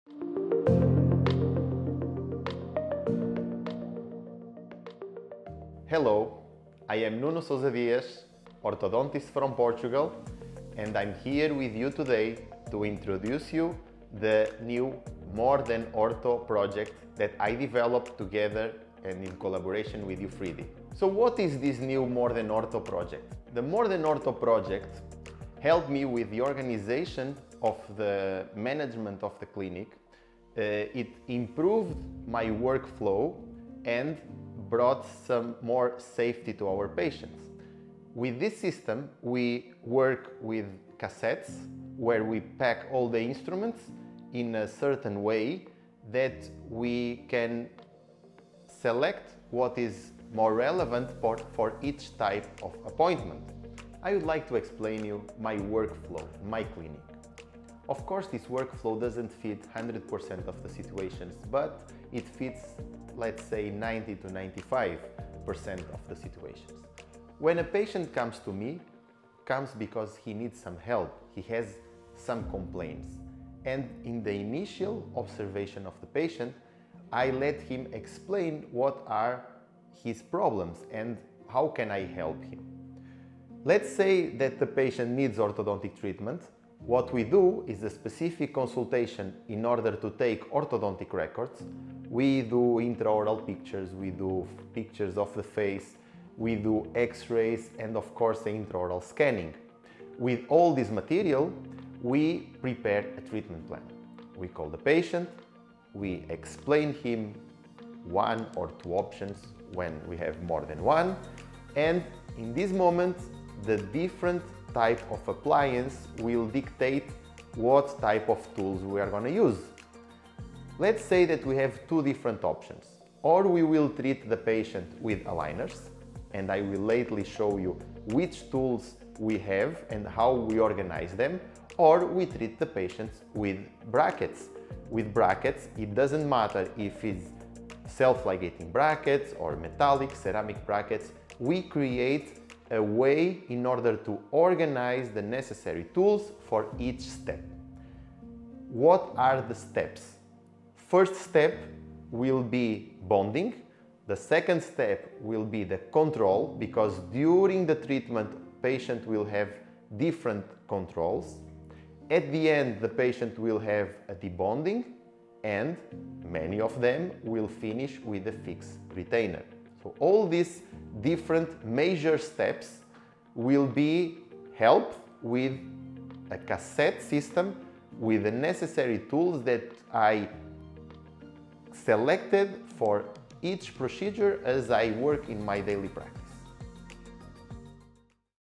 More Than Ortho: An Orthodontic Journey - with Dr. Nuno Sousa Dias
Be always ready and prepared throughout your orthodontic journey from the beginning until the end of a complete patient’s treatment.